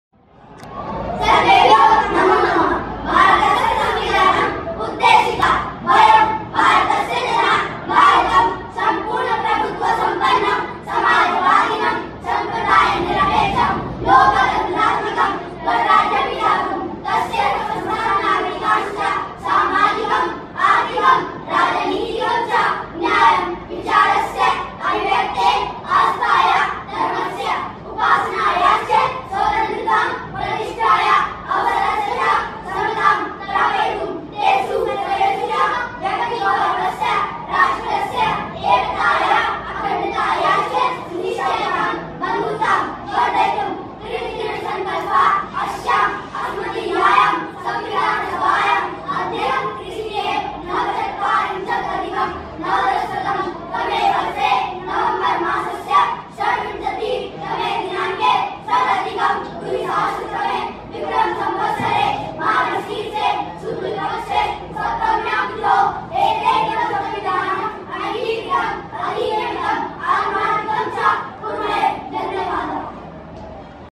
Preamble of Indian constitution in Sanskrit _ (भारतस्य संविधानम् _ उद्देशिका)
भारतस्य संविधानम्
उद्देशिका

वयम् , भारतस्य जनाः , भारतम् , सम्पूर्ण-प्रभूत्व-सम्पन्नम् , समाजवादिनम् , सम्प्रदाय-निरपेक्षम् , लोकतन्त्रात्मकम् , गणराज्यम् , विधातुम् , तस्य समस्तान् नागरिकाश्च ,

                      सामाजिकम् , आर्थिकम् , राजनीतिकम् च न्यायम् ,
                      विचारस्य , अभिव्यक्तेः , आस्थायाः , धर्मस्य , उपासनायाश्च स्वतन्त्रताम् ,
                      प्रतिष्ठायाः , अवसरस्य च , समताम् ,
प्रापयितुम् ,
तेषु सर्वेषु च ,

व्यक्ति-गौरवस्य , राष्ट्रस्य , एकतायाः ,
अखण्डतायाश्च , सुनिश्चायिकाम् , बन्धुताम् ,
वर्धयितुम् ,

कृत-दृढ-संकल्पाः , अस्याम् , अस्मदीयायाम् , संविधान-सभायाम् ,
अद्य , ख्रिस्तीये , नव-चत्वारिंशत्-अधिकम् नव-दश-शतम्–तमे–वर्षे नवम्बर–मासस्य षड्-विंशतिः–तमे-दिनांके ( षड्-अधिकम्-द्विहस्र-तमे विक्रम-सम्वत्सरे मार्ग-शीर्षे , शुक्ल-पक्षे , सप्तम्याम्-तिथौ )
एतेन इदम् संविधनम् , अंगीकृतम् , अधिनियमितम् , आत्मार्पितम् च ,

कुर्महे ।